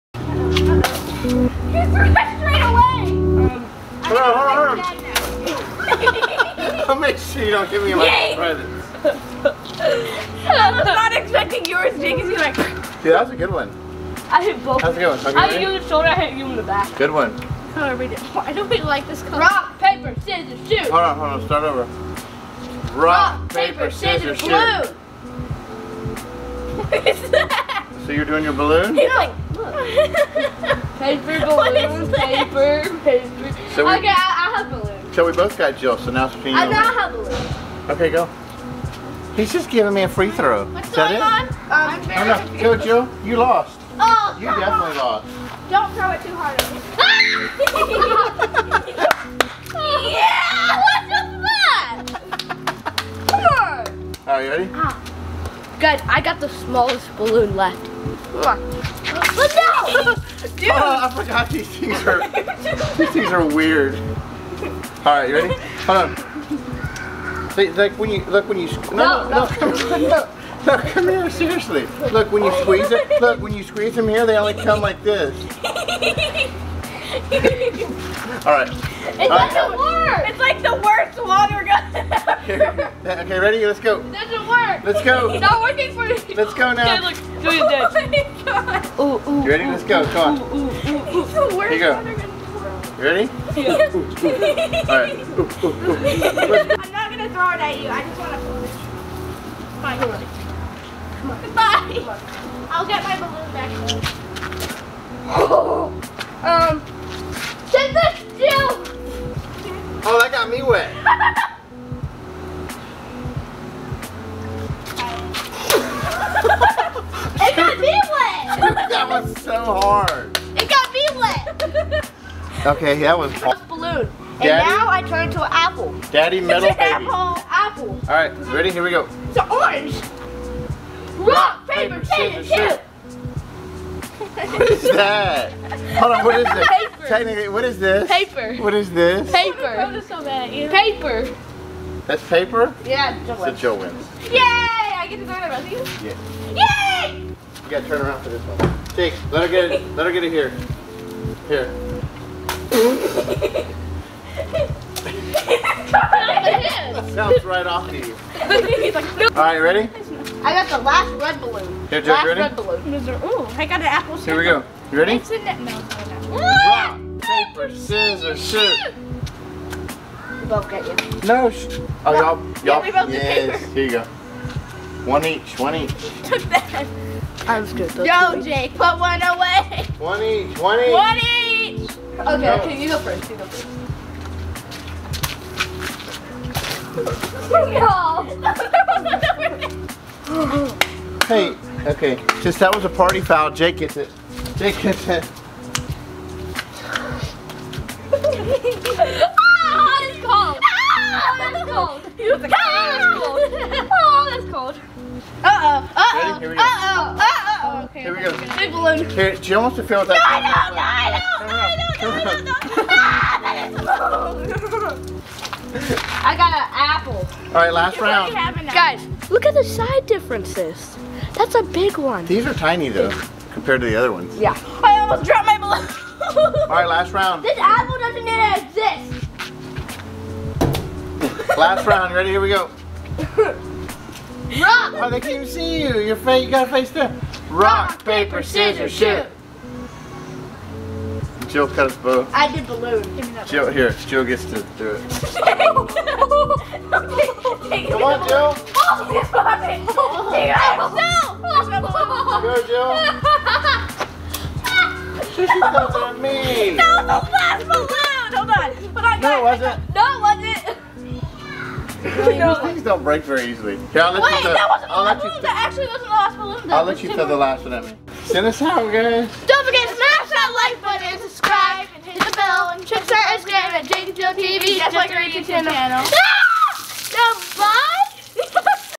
Paper Scissors Rock Game & Water balloons Challenge for Kids
Paper Scissors Rock is a very popular game with jake and jill and their dad.  In this video, they compete to see who is the best player by playing against each other. Watch them compete in this fun game and see who wins!